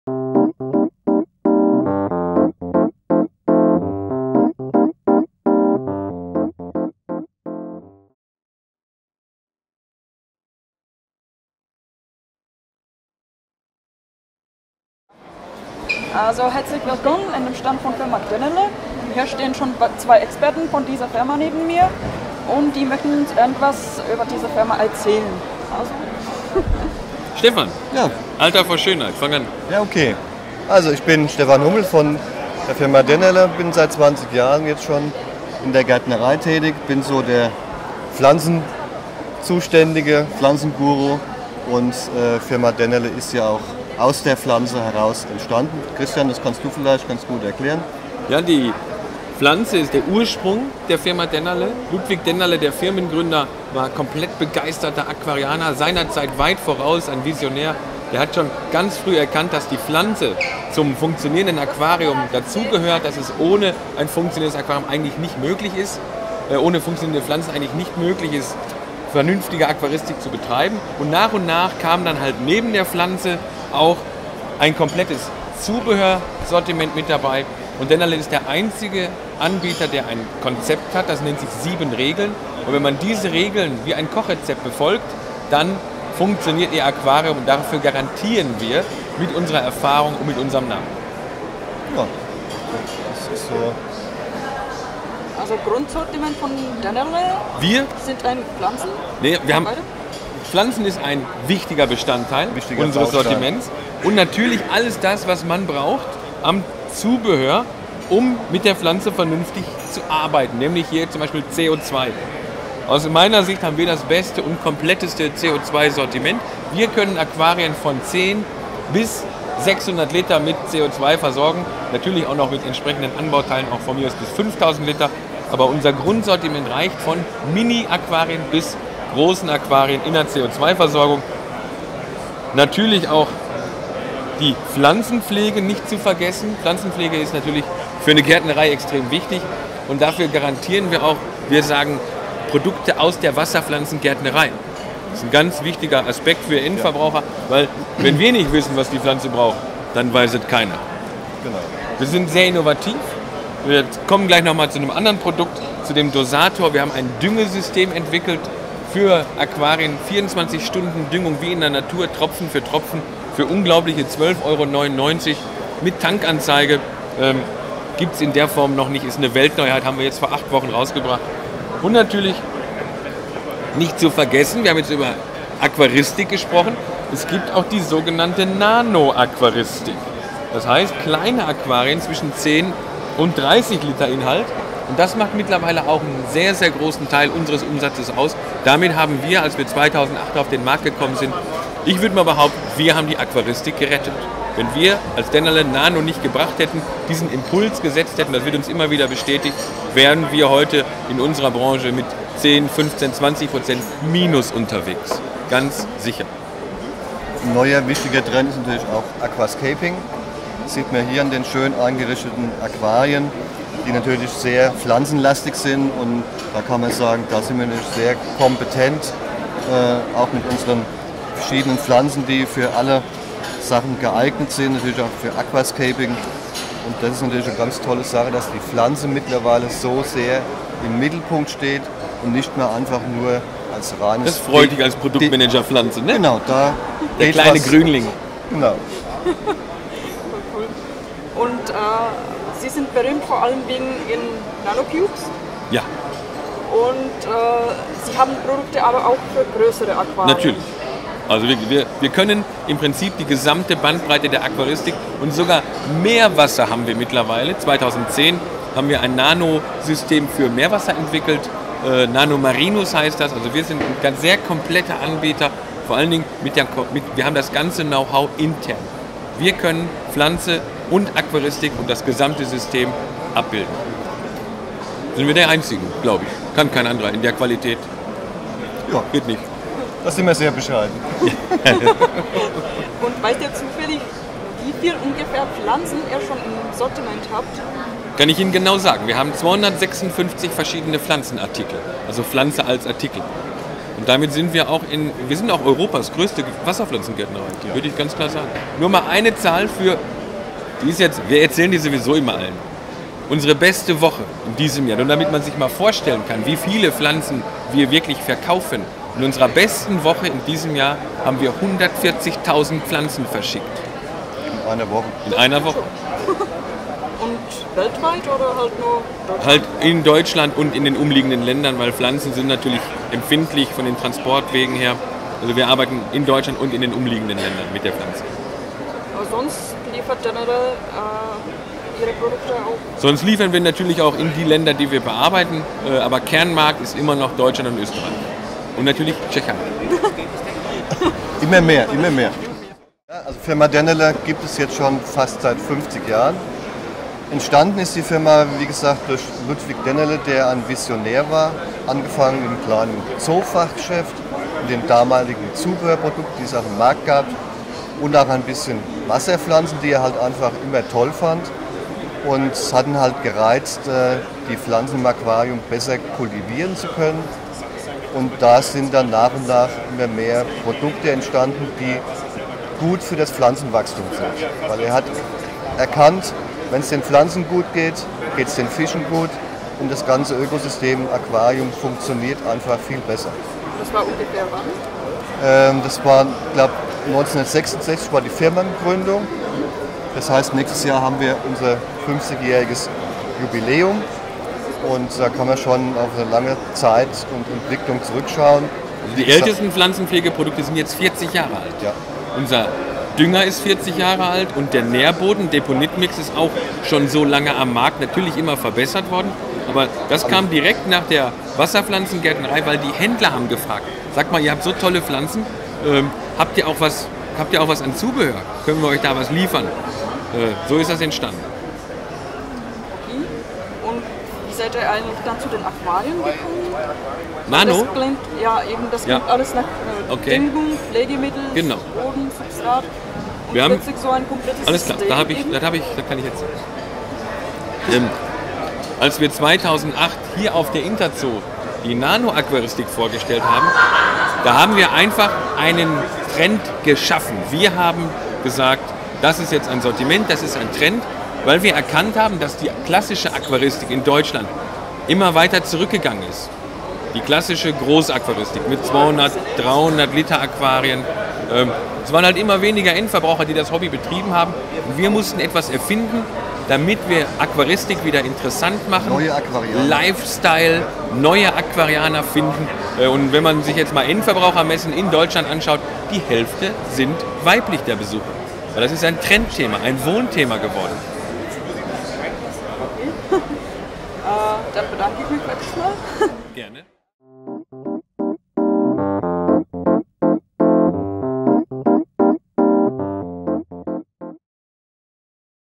Macenauer TV - Představení společnosti Dennerle (CZ titulky)
Společnost Dennerle vznikla před 50ti lety. Její zakladatel, Ludwig Dennerle, byl nadšeným akvaristou a vizionářem své doby. Pochopil, že akvarijní rostliny k fungujícímu akváriu nezbytně patří. Na základě toho postupně vznikaly další a další produkty podporující péči a růst rostlin.

V současnosti firma Dennerle patří k evropské špičce mezi výrobci v oblasti akvaristiky. Díky pestré nabídce prvotřídních akvarijních rostlin a příslušenství tak poskytuje kompletní program, který uspokojí i nejnáročnější akvaristy. Nabízí širokou nabídku sortimentu jak pro klasická akvária, tak i pro nanoakvária či aquascaping.

Výhradní dovozce do České a Slovenské republiky
Jaroslav Macenauer, Ing. - AKVARIUM
Bláhova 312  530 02 Pardubice - Ostřešany

Company Dennerle was founded 50 years ago. Founder, Ludwig Dennerle, was an enthusiastic aquarist and visionary. He realized that aquarium plants necessarily belong to the functional aquarium. On that basis gradually arose another products which support the care and growth of plants.

Nowadays company Dennerle belongs to the top of European manufacturers in the aquaristics. Thanks to wide variety of first-class aquarium plants and accessories provides a complete program which satisfies even the most demanding aquarists. Dennerle offers a wide range of assortment for classic aquariums, nanoaquariums or aquascaping.

Exclusive importer to the Czech and Slovak Republic